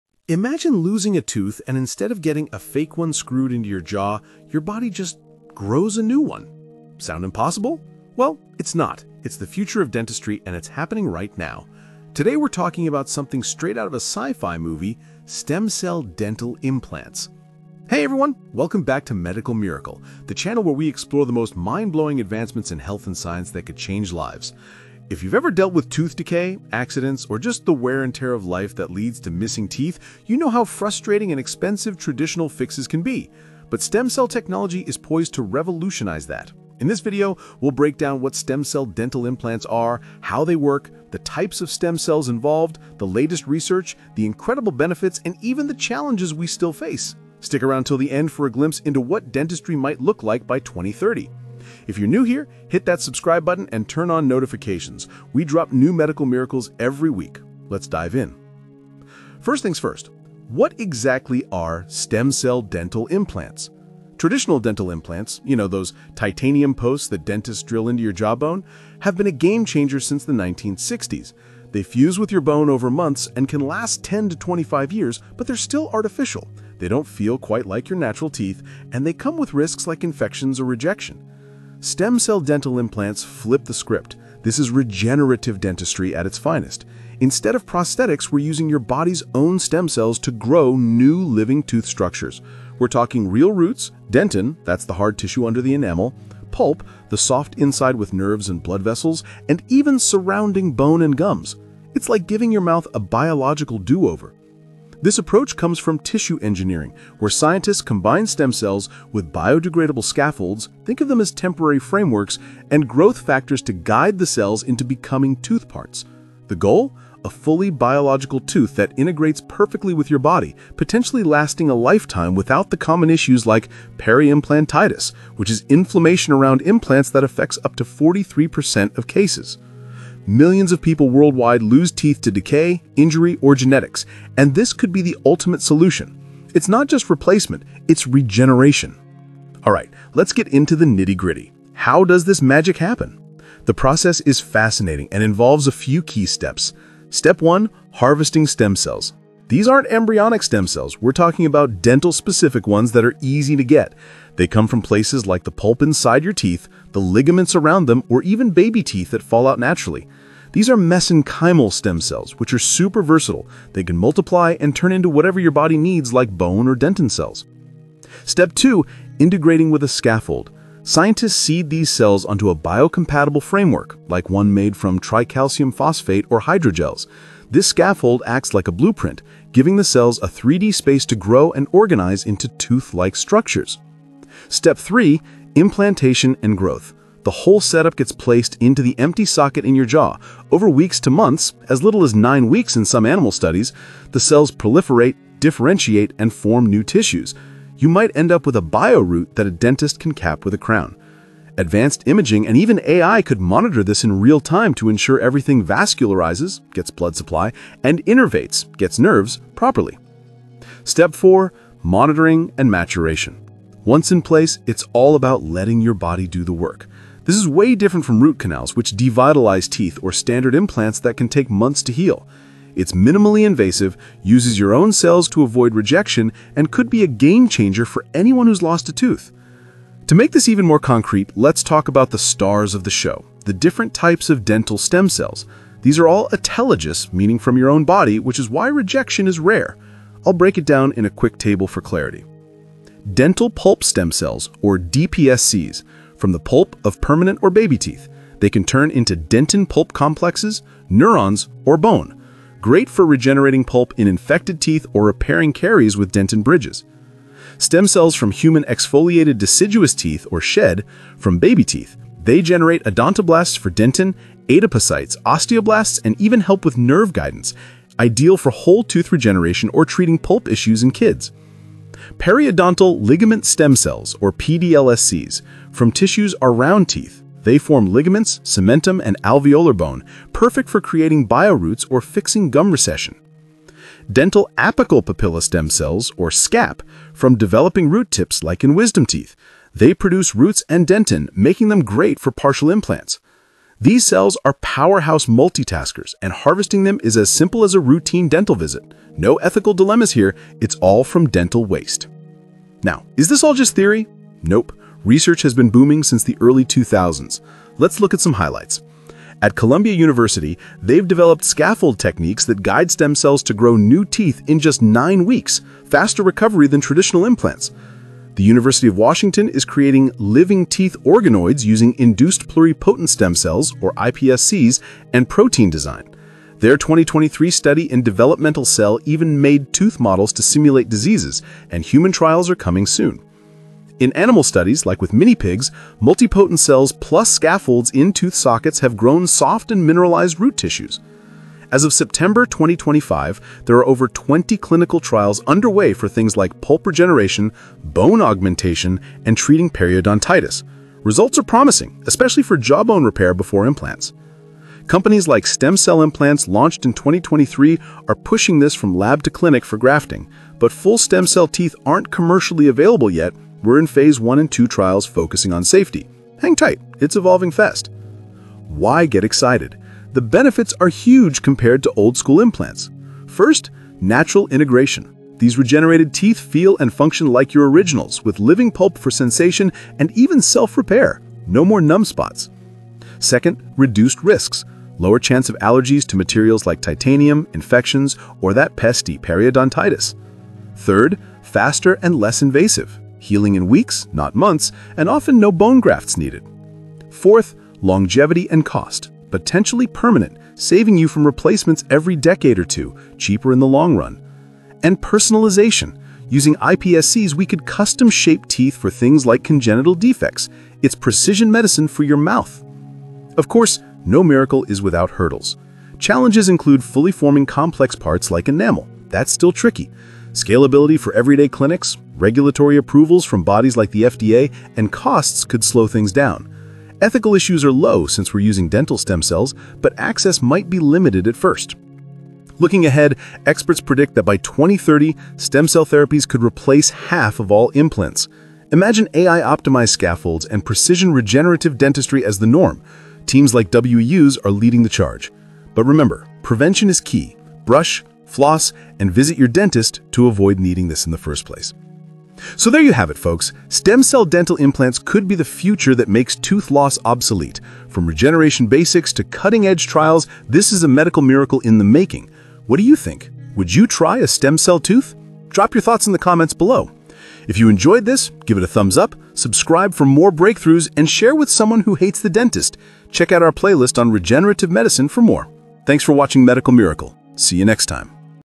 Stem Cell Dental Implants The Future of Dentistry | Medical Miracle
In this groundbreaking episode of Medical Miracle, discover how stem cell dental implants are revolutionizing dentistry and could make tooth loss obsolete. We explore the science behind using stem cells to regenerate natural teeth, including harvesting from dental pulp, scaffolds for growth, and the step-by-step process of creating bio-roots that integrate seamlessly with your body. Dive into the key types of dental stem cells like DPSCs and SHED, and learn about current research, such as next-generation designs that stimulate bone regeneration for implants in challenging sites.

We'll cover the benefits over traditional titanium implants—like faster healing, reduced infection risks, and lifelong natural function—plus challenges and future prospects, including predictions that stem cell therapies could dominate by 2030. Whether you're facing dental issues or just love cutting-edge medical tech, this video breaks it all down with visuals, tables, and expert insights from 2025 advancements.
Timestamps:
00:00 - Intro to the Future of Dentistry
01:00 - What Are Stem Cell Dental Implants?
02:00 - How They Work: Step-by-Step
03:30 - Types of Dental Stem Cells
04:30 - Latest Research & Trials (2025 Updates)
05:20 - Benefits vs. Traditional Implants
6:00 - Challenges & What's Next
7:30 - Conclusion & Your Thoughts